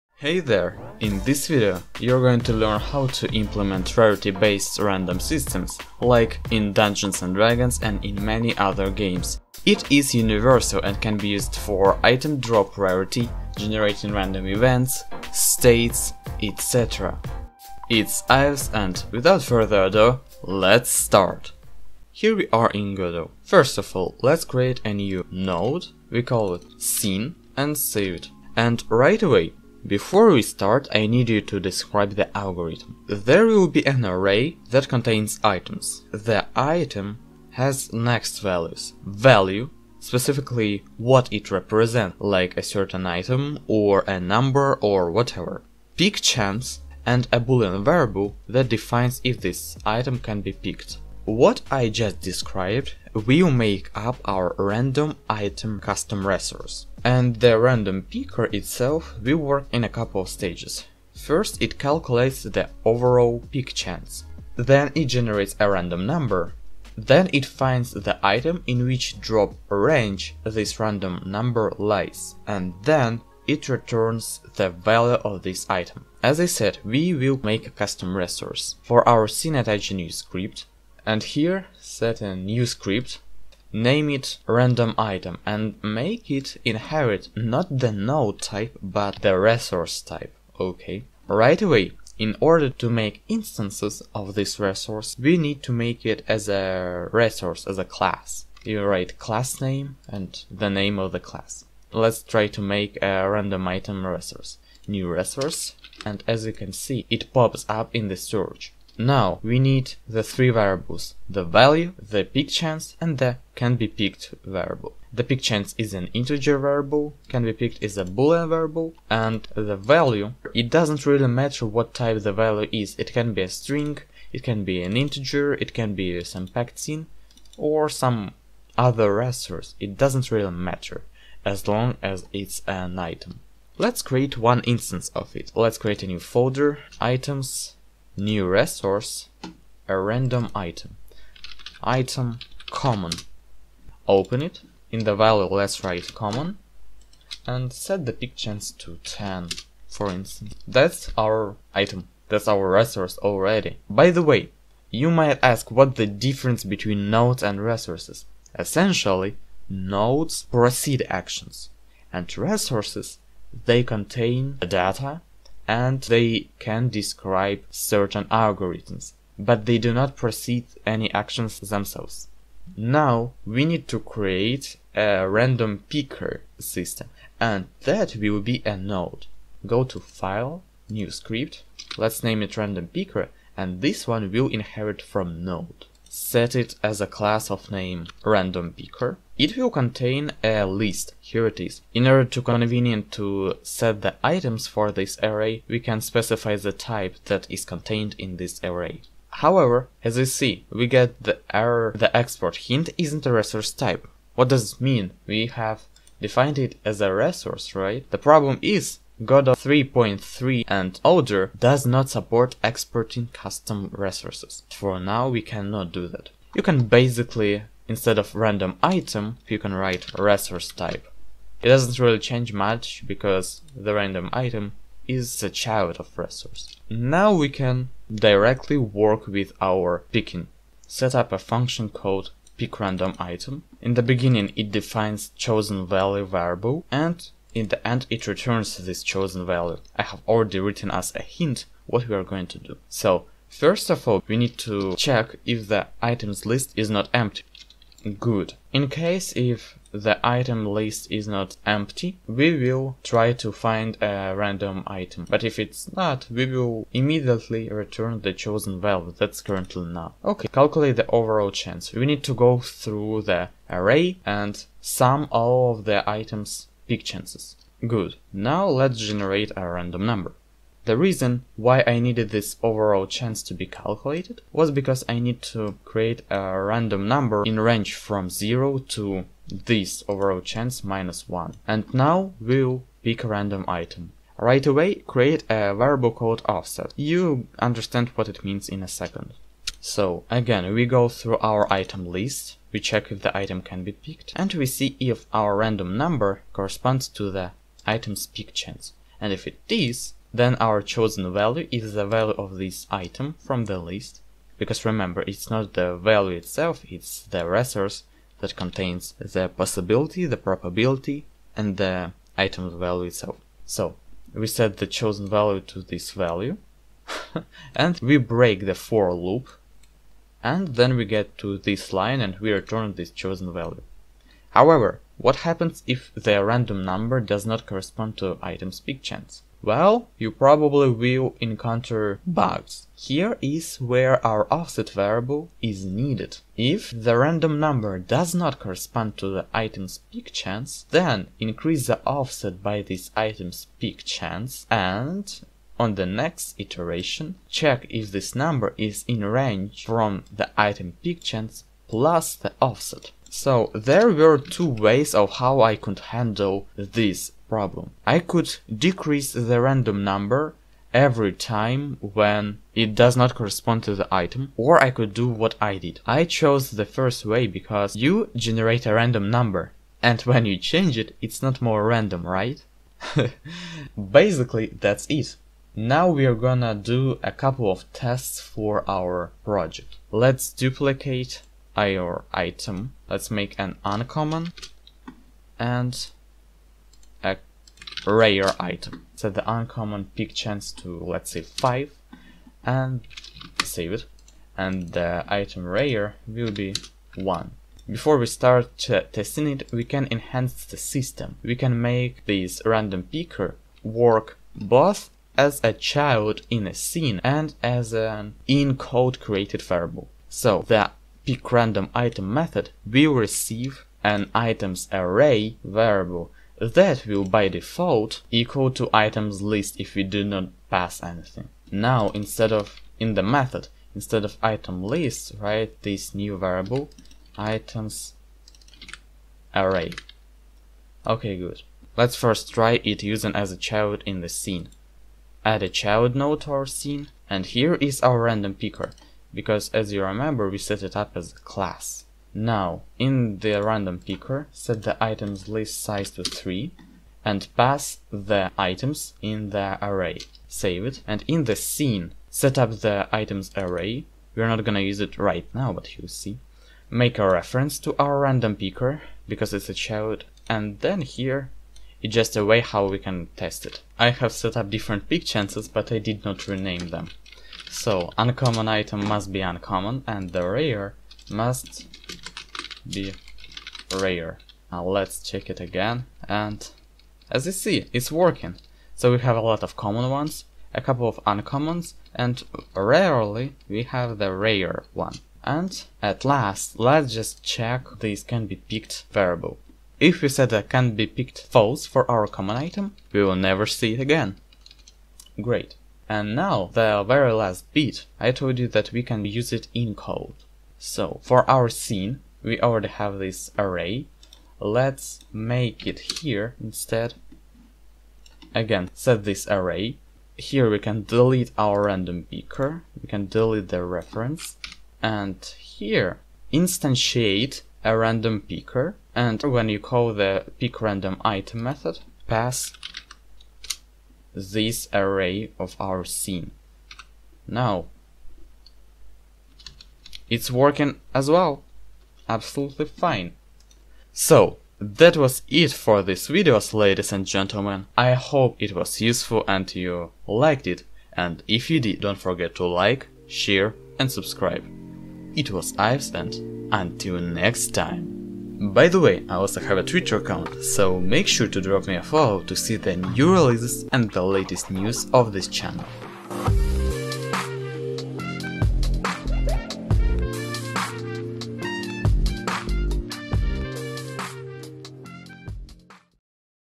Chance-Based Random Picker in Godot!
In this video, I'll teach you to make a random picker system that supports different probabilities of each item (rarity feature) and can be used for virtually any purpose, from the basic item drop to some complex event picker system!